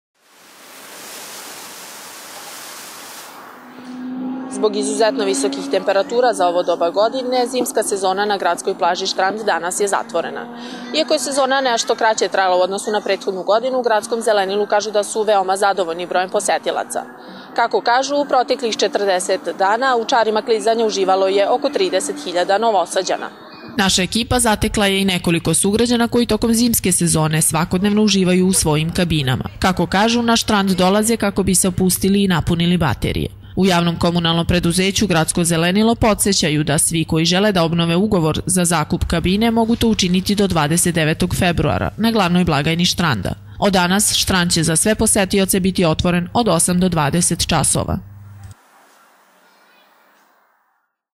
TV KANAL 9, NOVI SAD: ZATVORENA ZIMSKA SEZONA NA ŠTRANDU
Proteklu zimsku sezonu na Štrandu obeležile su brojne aktivnosti i humanitarne akcije, kažu u JKP "Gradsko zelenilo". Posete su premašile sva očekivanja, a tokom mesec i po dana iznajmljeno je preko 11 hiljada klizaljki. Kroz školu klizanja prošlo je oko tridesetak mališana.